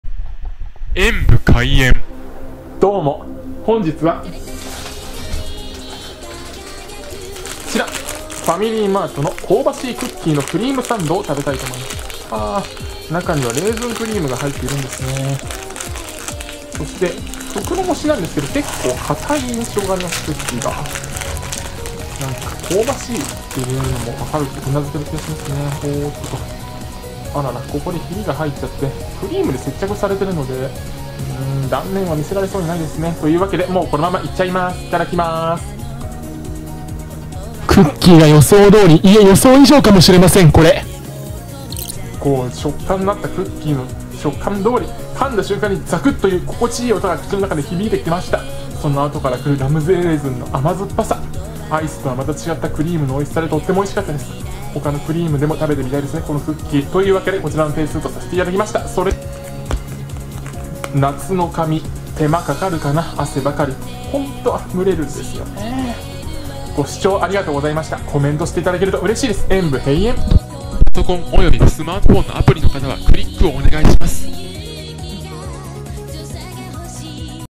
ファミリーマート 香ばしいクッキーのクリームサンドを食べてみた。
ファミリーマート　香ばしいクッキーのクリームサンドを食べてみました。触感からもう香ばしさが伝わってきます此のクッキー、音も良い～
贈り物は此方まで↓
〒153-0042